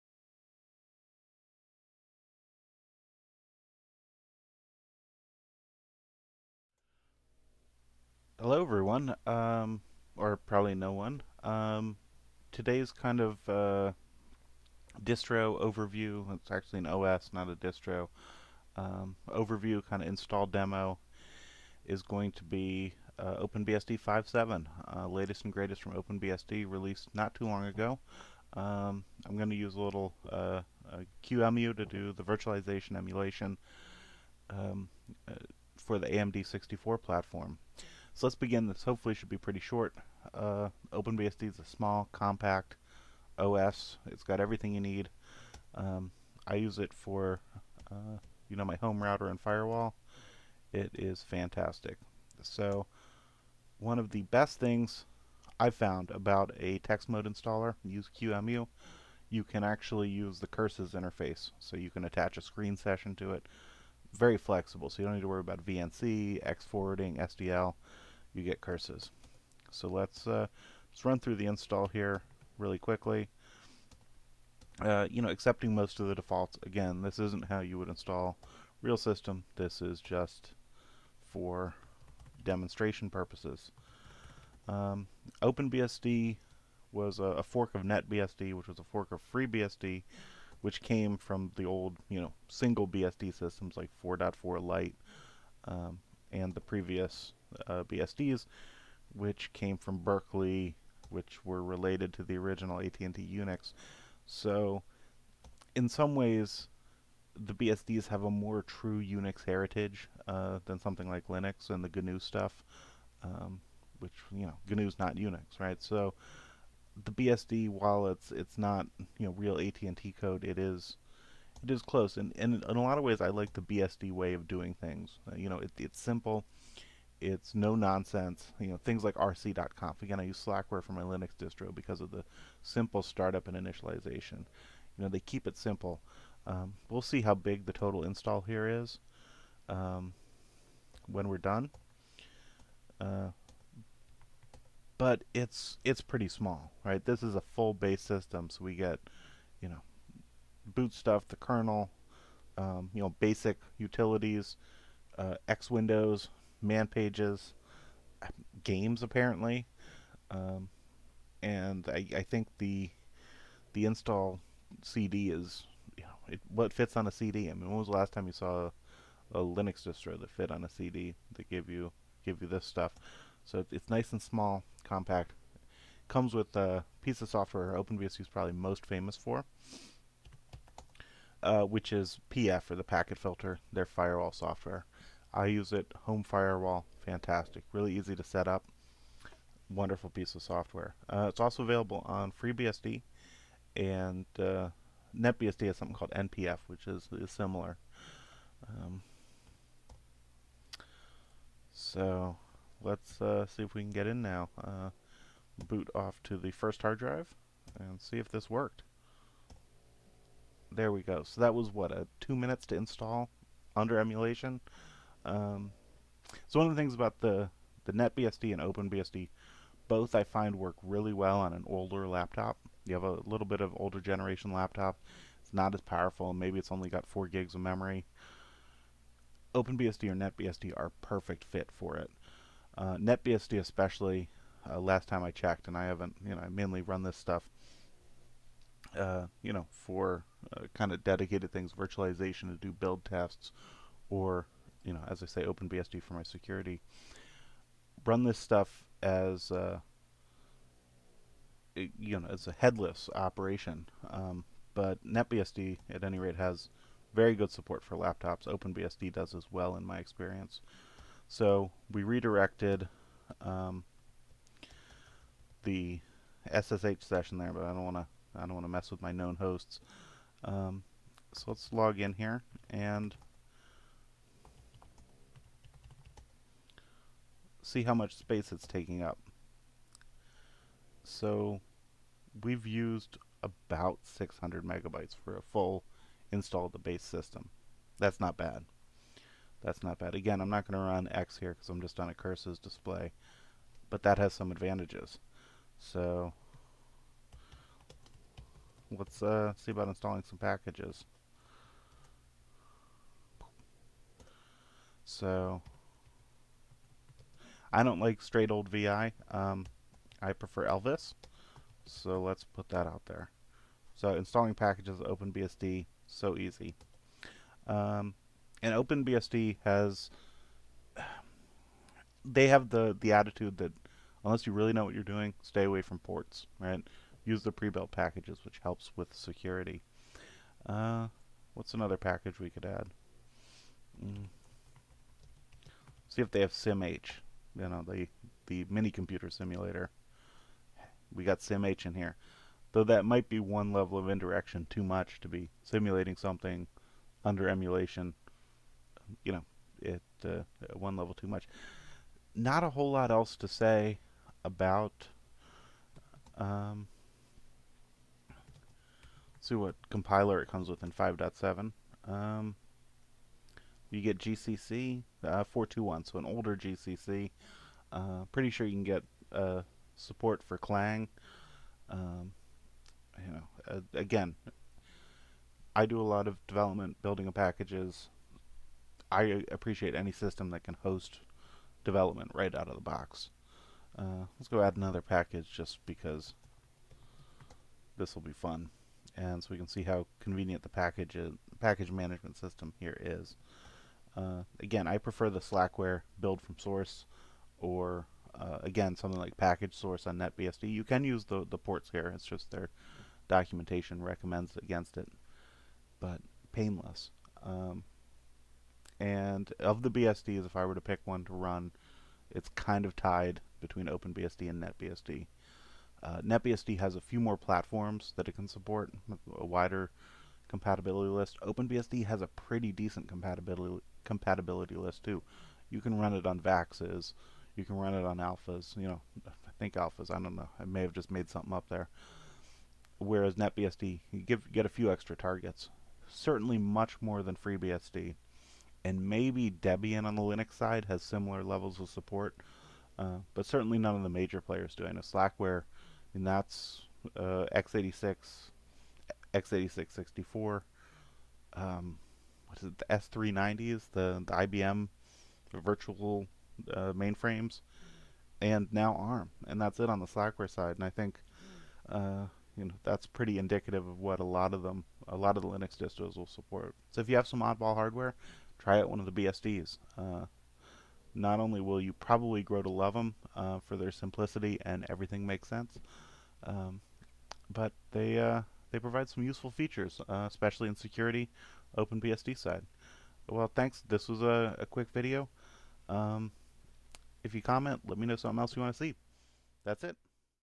Open BSD 5.7 Review/Overview
I drone on as I install Open BSD 5.7 as a qemu guest